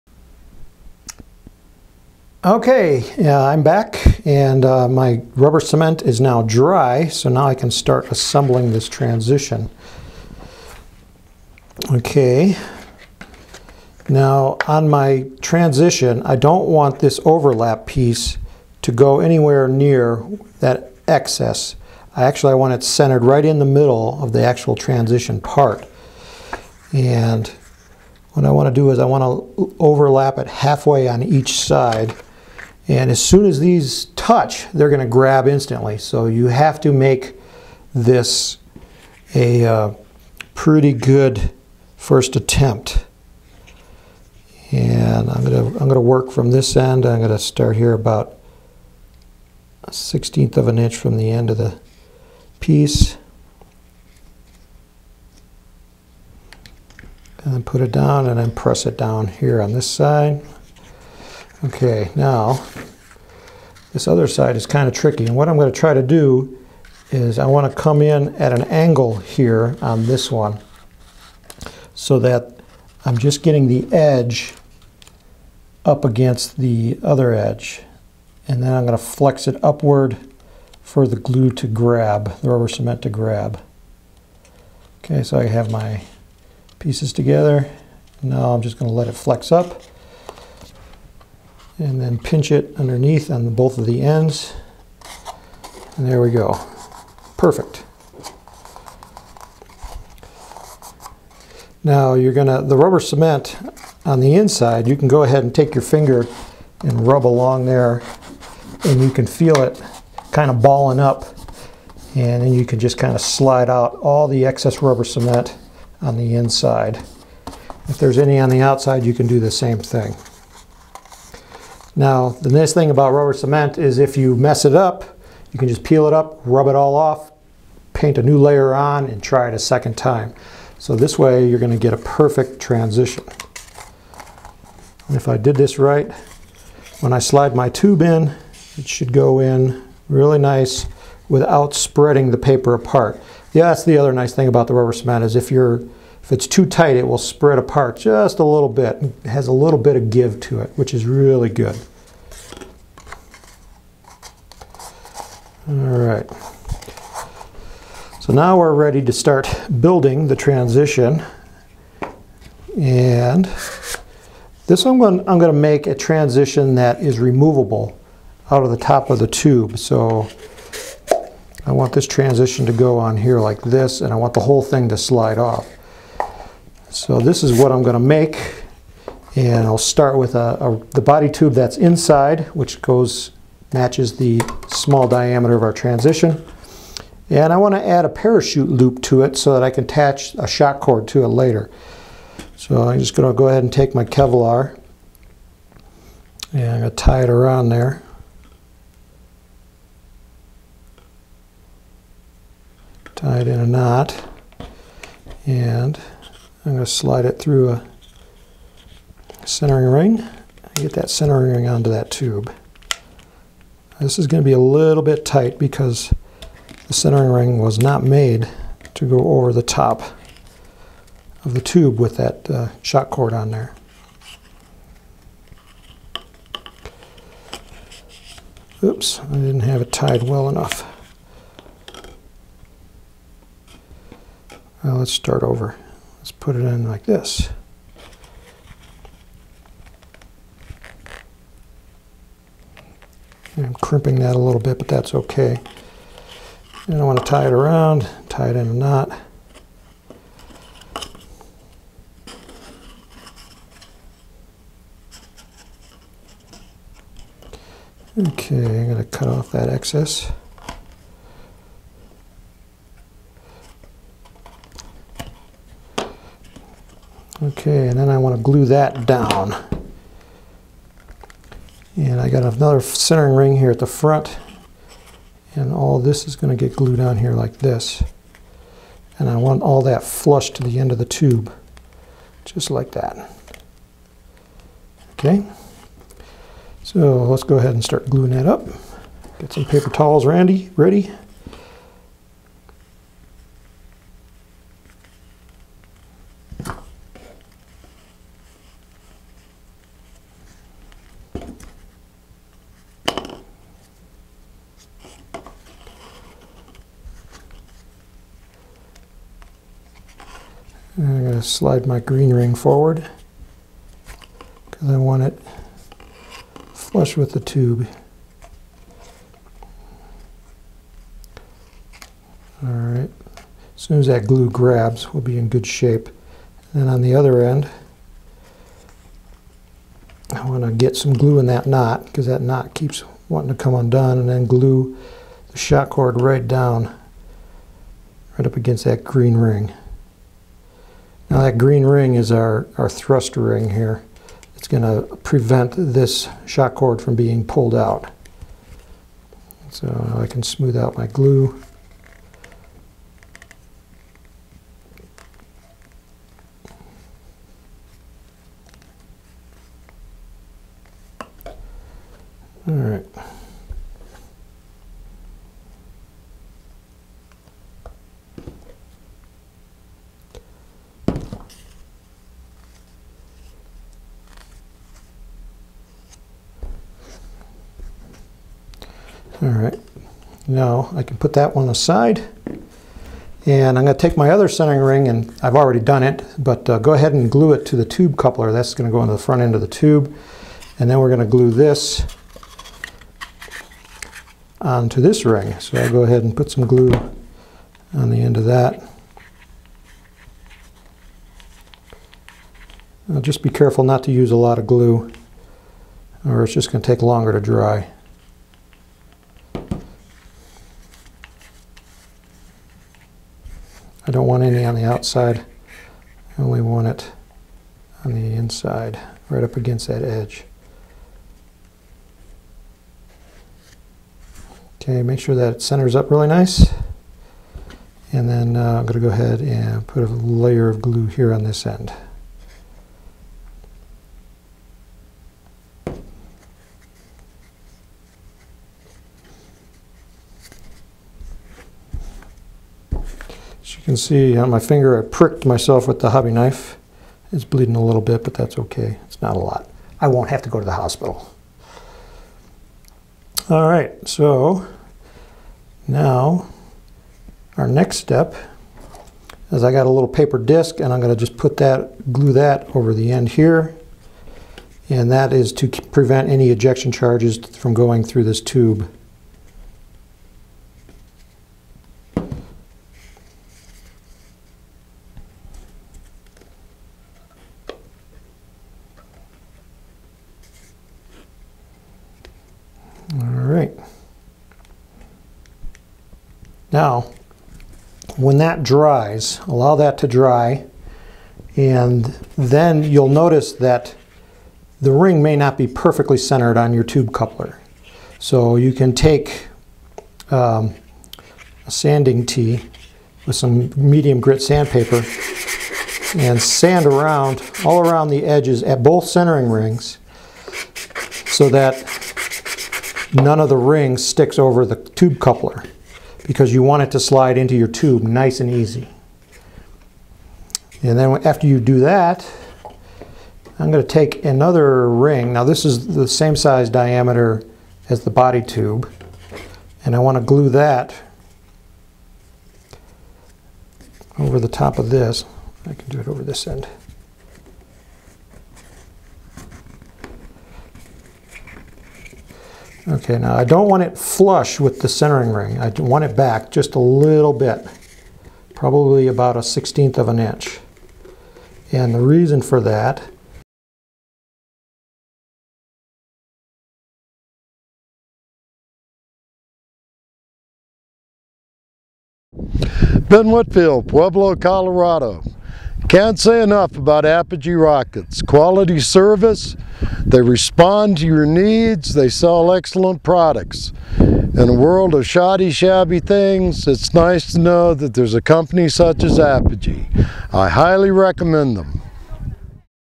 How to Make A Paper Transition for Model Rockets - Part 3
Making a paper transition section for a model rocket - Part 3. Here you'll see the secret glue that I use to get a perfect curve on the outer surface of the transition. Then we'll start building the transition section structure so that you end up with a strong rocket